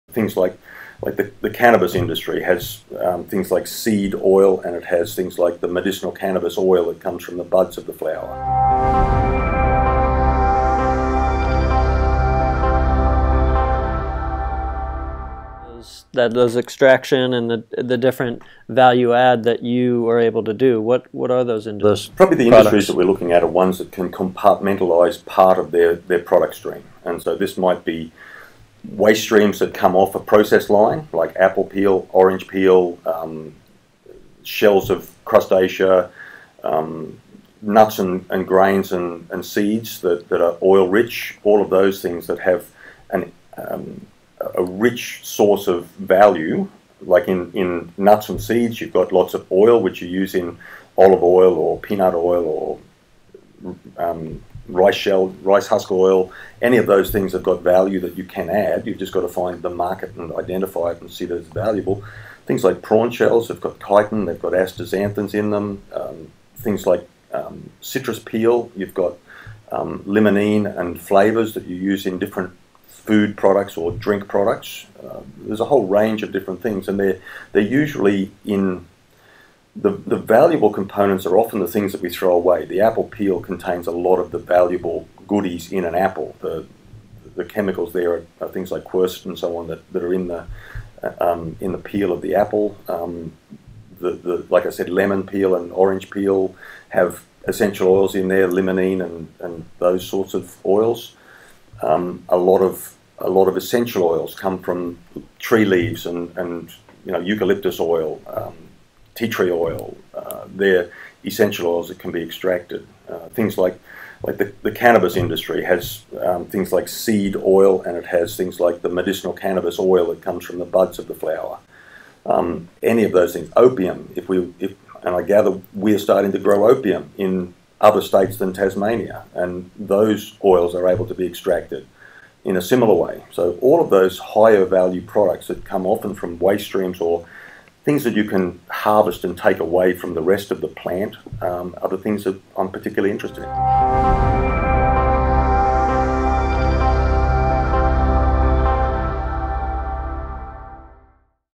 Cannabis Oil
Cannabis oil developed in Australia. Craig and Joshua discuss cannabis oil extract. How that is being conducted? Cannabis oil, cannabis fibre, cannabis seed.